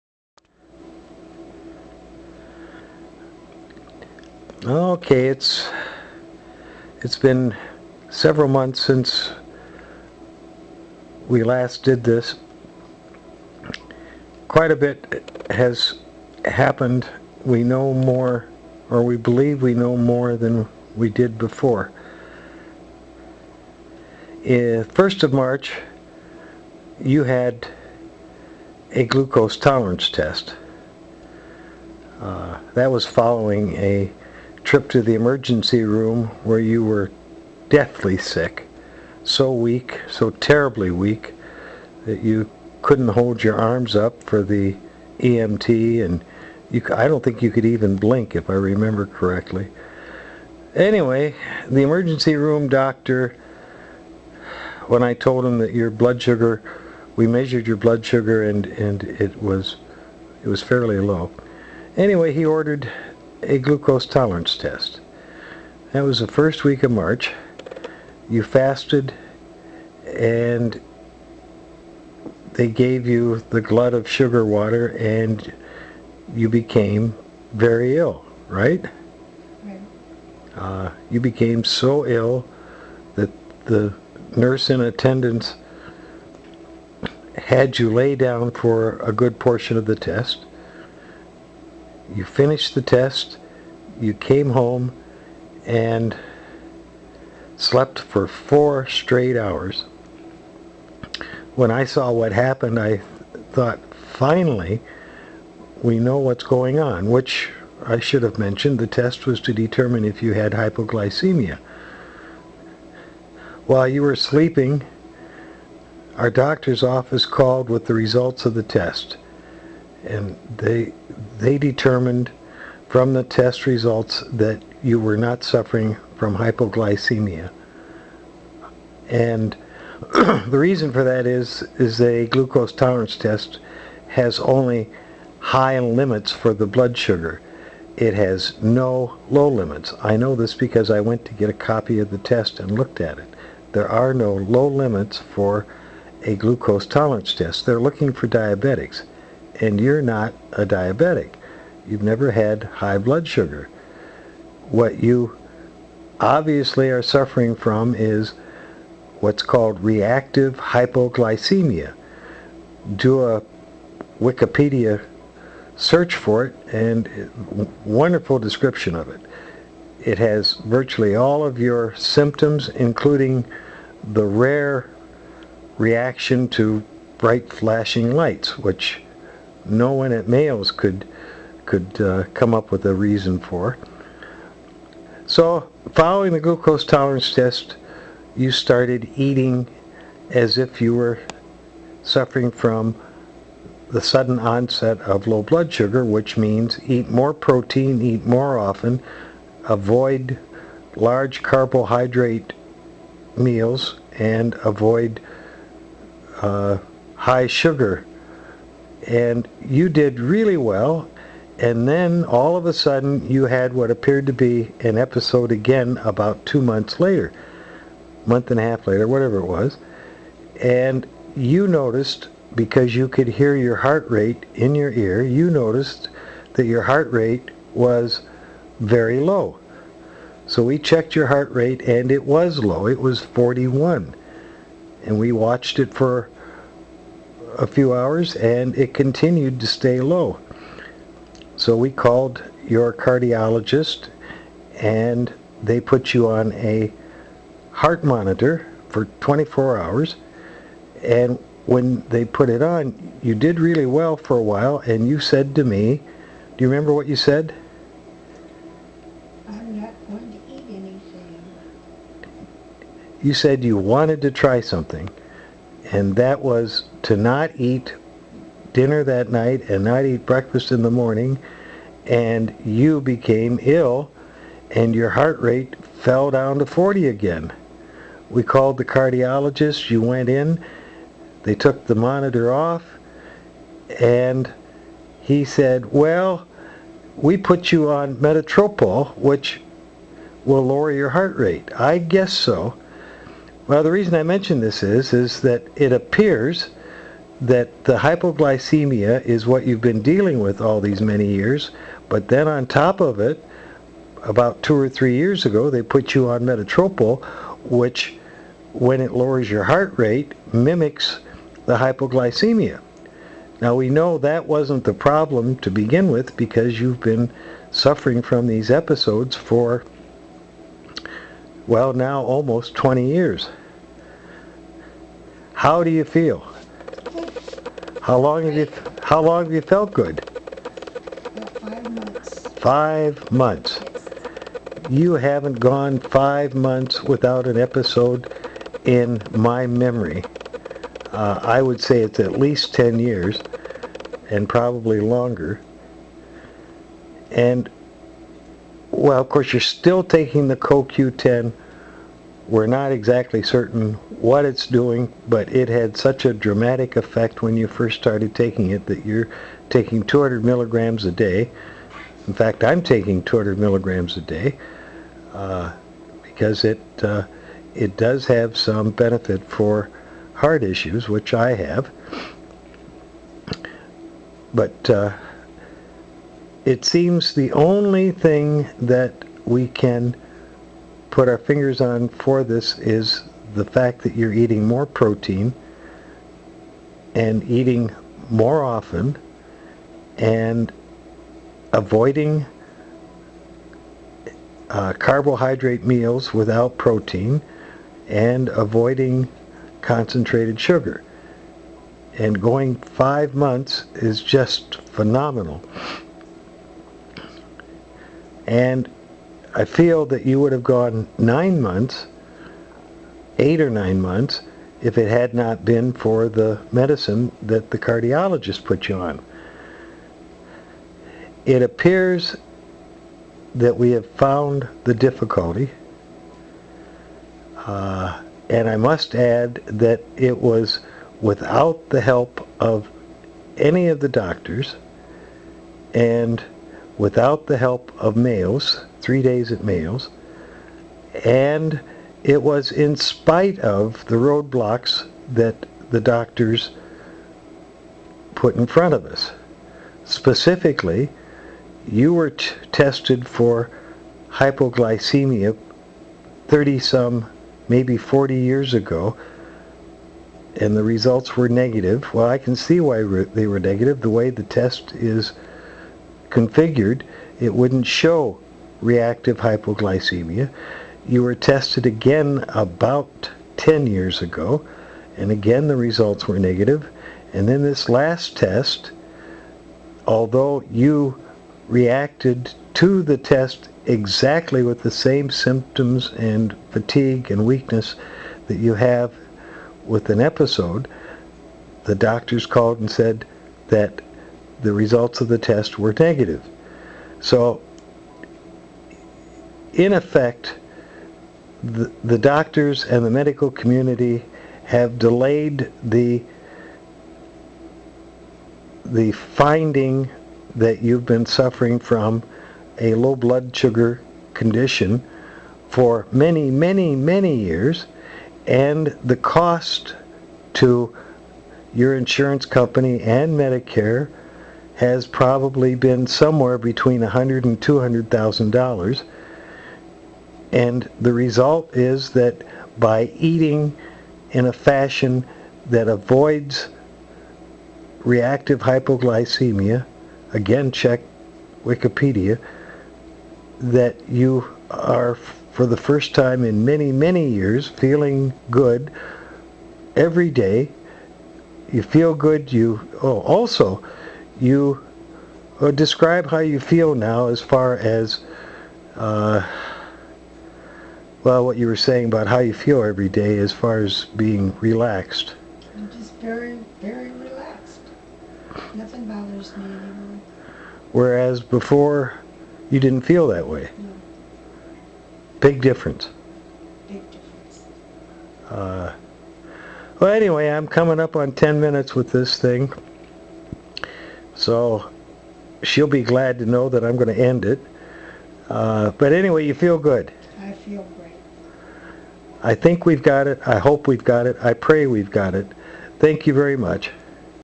We are convinced that it is reactive hypoglycemia
Mary has now gone over 5 months without an "episode"--that is the longest that she has gone in over 10 years--In spite of all the doctors and after $100,000 to $200,000 cost to  insurance and medicare.  WE have found the source of the sudden onset of extreme weakness and fatigue--REACTIVE HYPOGLYCEMIA--look it up on wikipedia "reactive hypoglycemia"